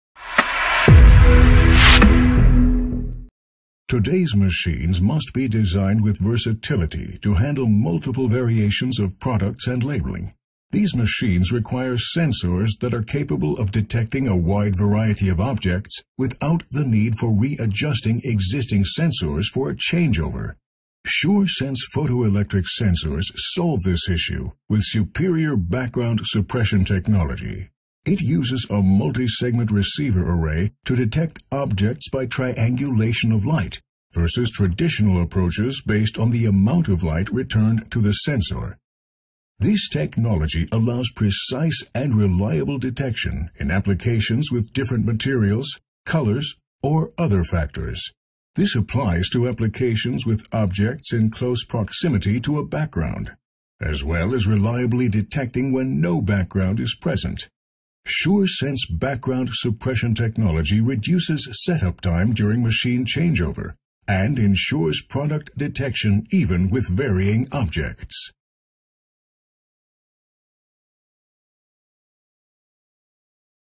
Cisimden Yansımalı Fotoelektrik Sensör
Tüm cisimden yansıma fotosel sistemleri için uygundur.
Kolay montaj ve sağlam bağlantılıdır
İrtibat için adresinden bilgi alabilirsiniz.